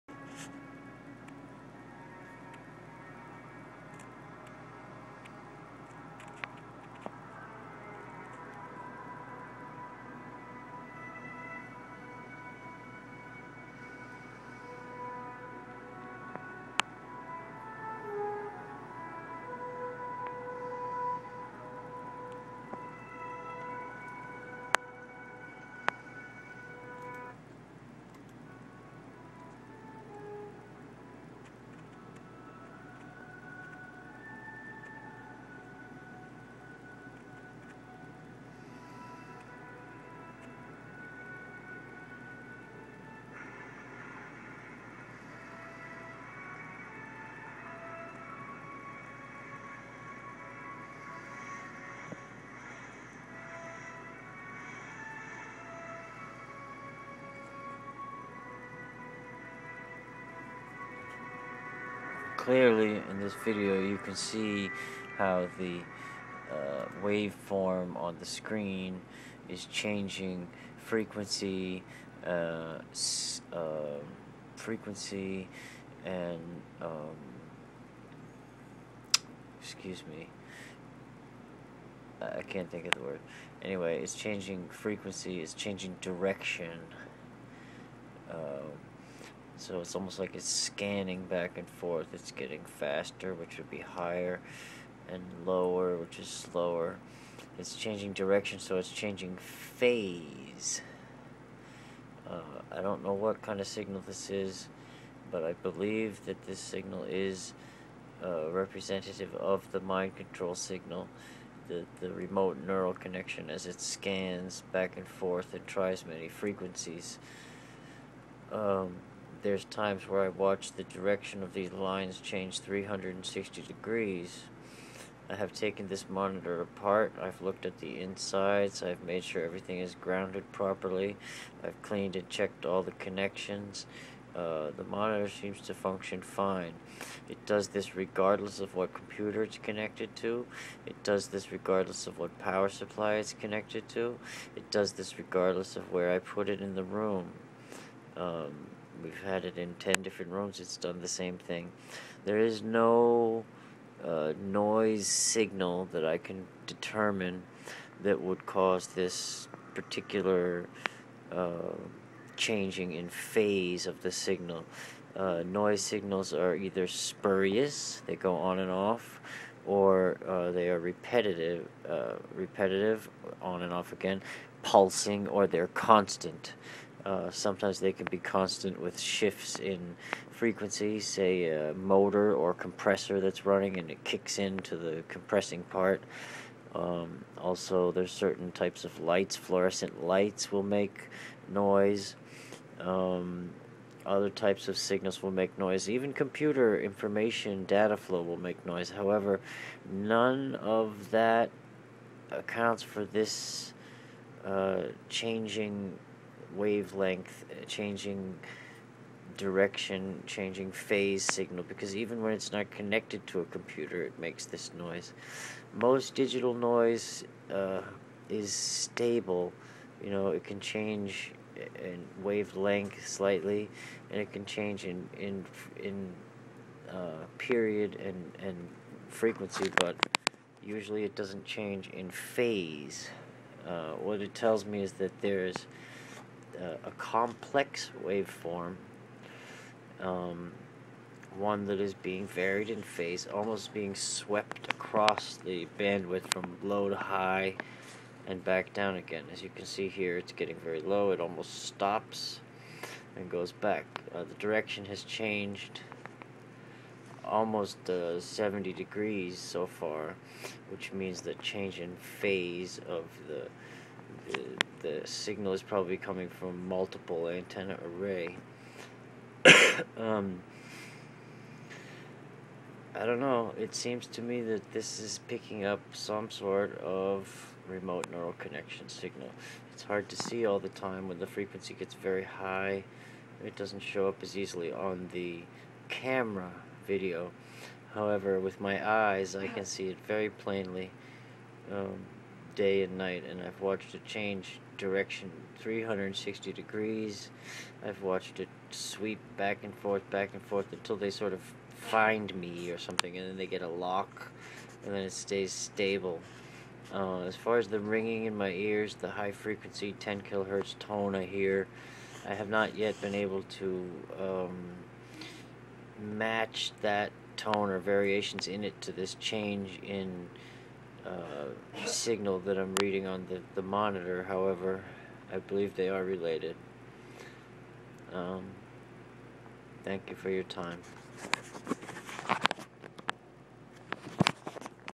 Timothytrespas: remote neural (mind control) rf signal detected on LCD monitor
Timothytrespas: remote neural (mind control) rf signal on monitor
THIS IS THE HARDEST THING TO RECORD TO VIDEO ON MY IPHONE IN HD!
WHY?
 I have tried to record this over and over from all angles at different settings on the monitor. it is just hard to record. but here in person, I CAN SEE LINES SWEEPING CHANGING PHASE AND SWEEPING FREQUENCY. You tell me.
I know it looks like i am just "crazy"and making this up, but if you believe anything I have said, believe me on this.
I can see the lines sweeping back and forth.
(so can Petra, I checked it out to make sure I was not seeing things.) so,without further comment, here we go:

 I can see a visual representation of what I believe to be the remote neural connection used for my mind control and torture by a secret section of the us government. I can find no reason other for the waveforms I see on this screen. Phase change and sweeping patterns that do not match any signals I can measure inside the monitor. A fluke, but there it is. My computers all pick up a similar signal that I can't really account for as purely digital ( or even analog) noise signal. It's a bit hard to see on the video but by eye it is clear as day. My attention is drawn to it as I receive voice to skull transmitting and mind control torture. Don't believe it? Then you have not done the research that would clearly show you that there are thousands of victims worldwide that call them selves targets. Gangstalking and mk ultra are other terms to search. For targets who suffer daily from this mind control experimentation, I know there is little doubt in their minds that this indeed exists and is happening to them. Global mind control is here now and being used on unsuspecting populations as you watch this video. Blessings and peace.